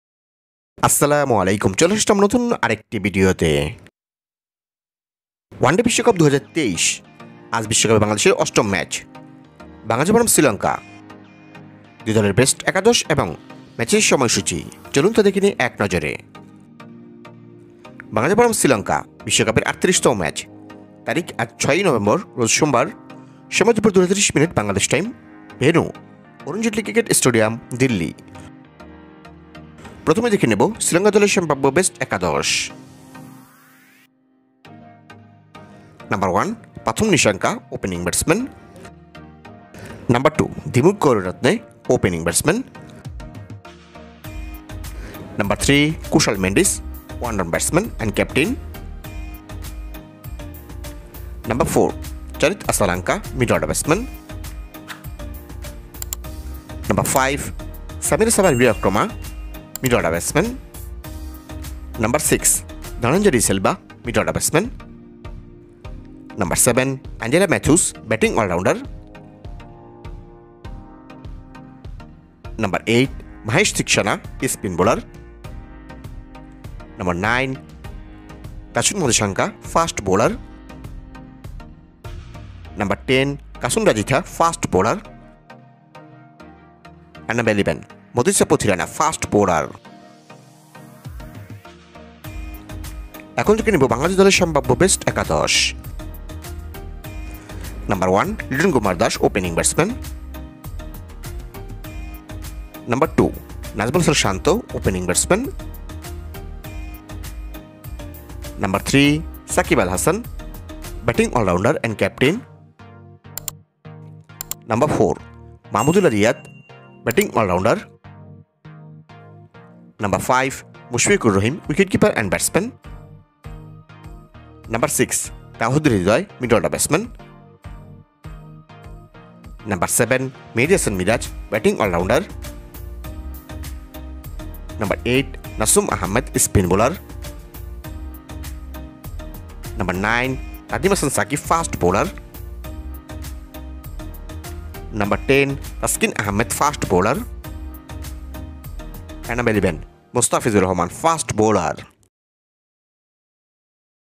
আজ জিতলেই নিশ্চিত বাংলাদেশের চ‍্যাম্পিয়ান্স ট্রফিতে অংশগ্রহণ | Bangladesh Vs Sri Lanka | World Cup
Welcome My Sm1 Sports Channel